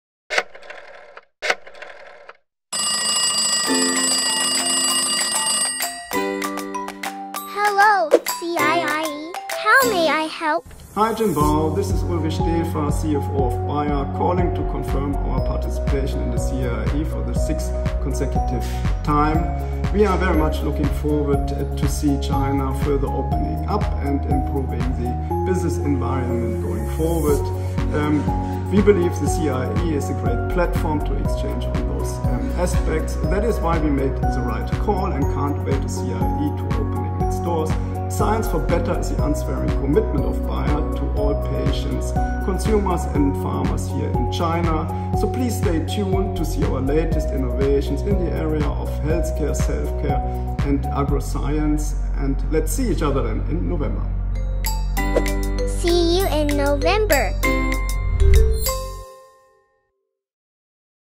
Bayer invites you to attend the 6th CIIE
🍀 🤙 @BayerGlobal is scheduled to make its 6th trip to the CIIE in November and showcase its latest innovations at the expo. Why has the German company been a part of every edition of the CIIE? Find out the answer from Ulrich Stefer, CFO of Bayer Greater China, in this video.